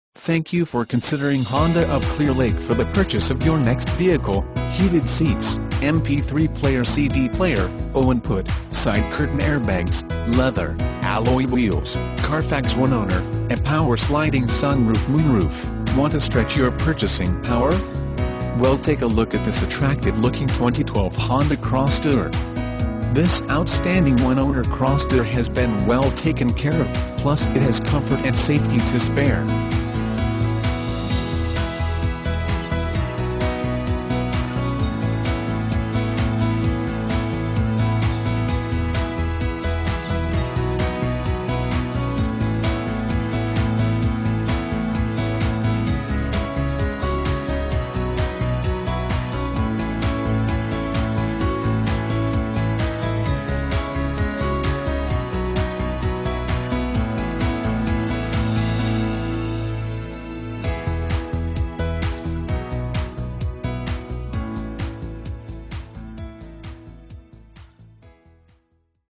2012 HONDA CROSSTOUR Webster, TX CL00122P
2012 HONDA CROSSTOUR Webster, TX
Stock# CL00122P

THANK YOU FOR CONSIDERING HONDA OF CLEAR LAKE FOR THE PURCHASE OF YOUR NEXT VEHICLE,. HEATED SEATS. , MP3 PLAYER/CD PLAYER. , AUX INPUT. , SIDE CURTAIN AIRBAGS. , LEATHER. , ALLOY WHEELS. , CARFAX ONE OWNER. , and POWER SLIDING SUNROOF/MOONROOF. Want to stretch your purchasing power? Well take a look at this attractive-looking 2012 Honda Crosstour. This outstanding one-owner Crosstour has been well taken care of, plus it has comfort and safety to spare.